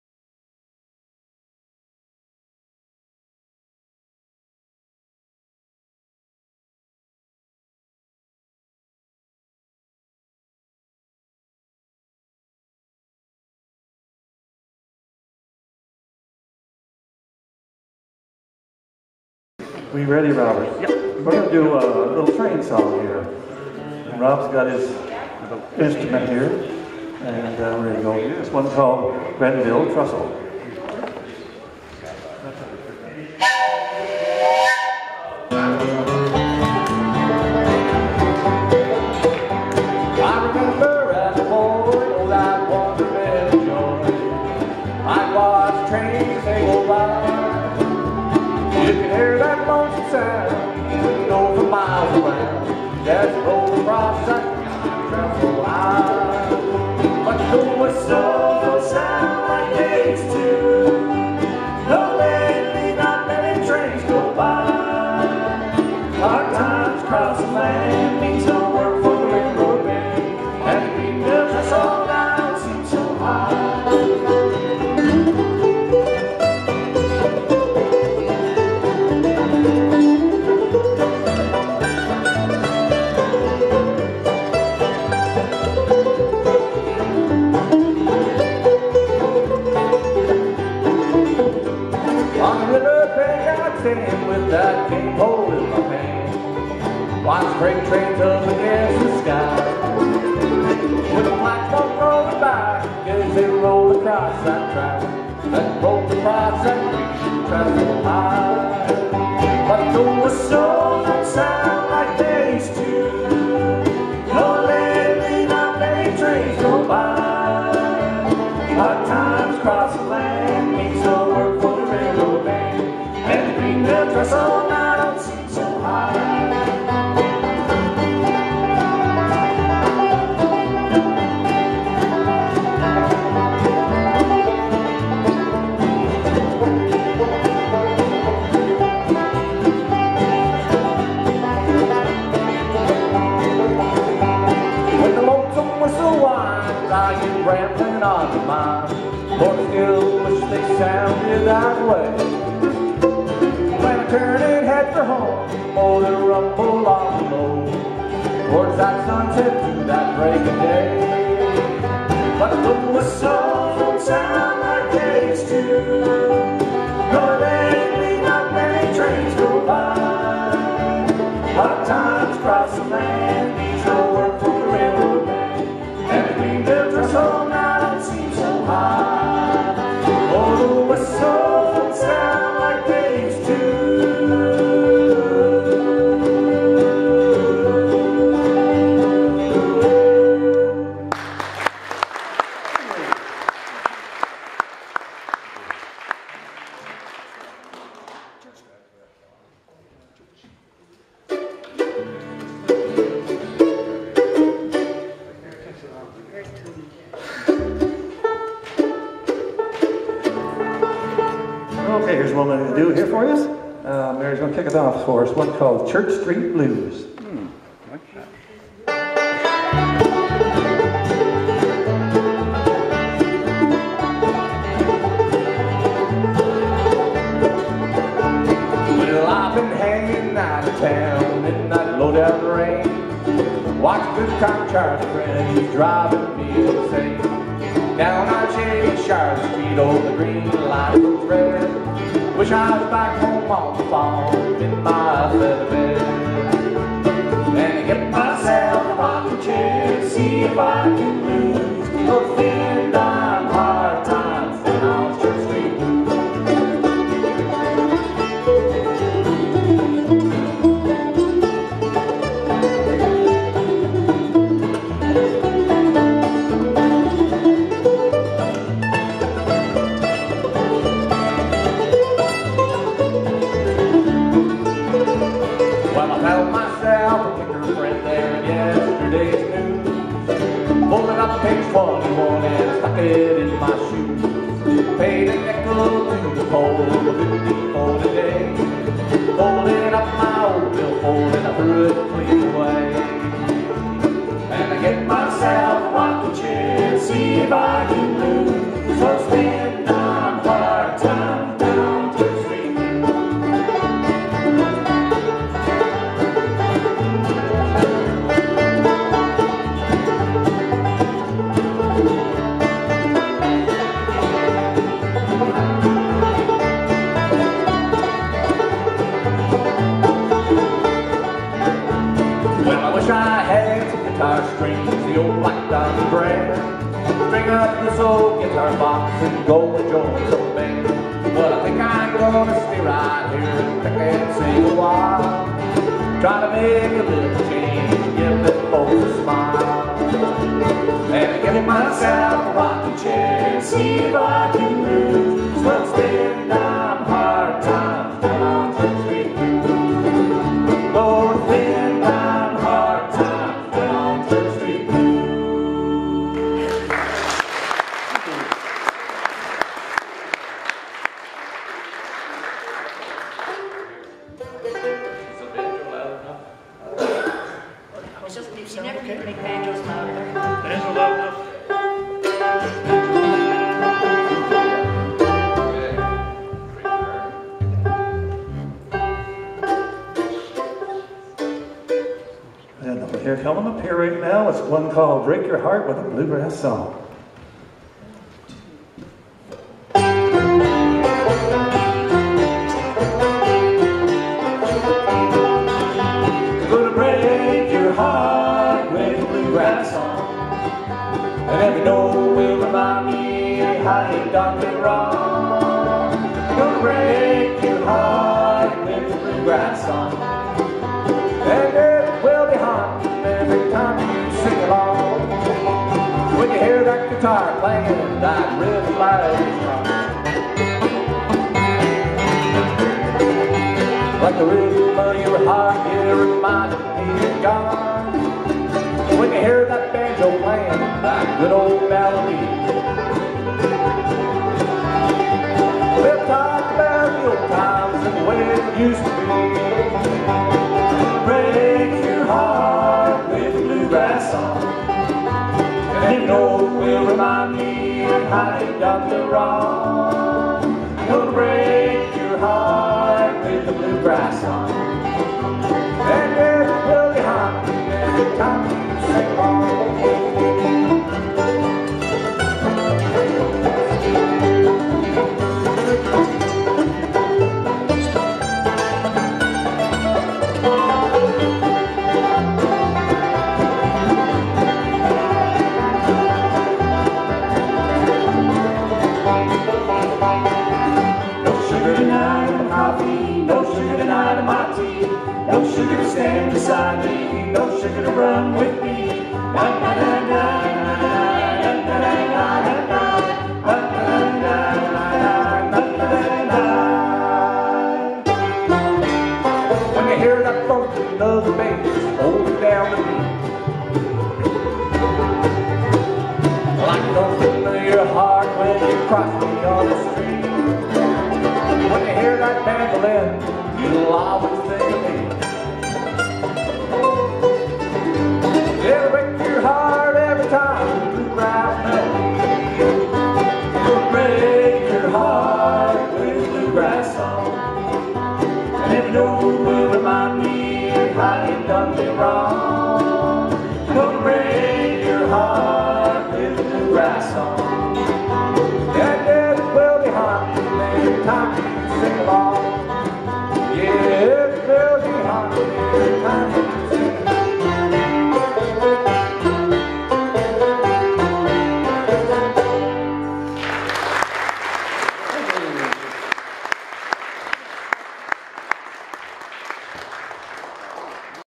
The High Strung Drifters...Odessa, 2014
Bluegrass. Recorded at the "Emanuel United Church", Odessa, Ontario. Musicians are: Cam Gilmour, Flattop & lead vocals; Mary Pearson, banjo; Rob Unger, Mandolin: Ken Gilmour, bass!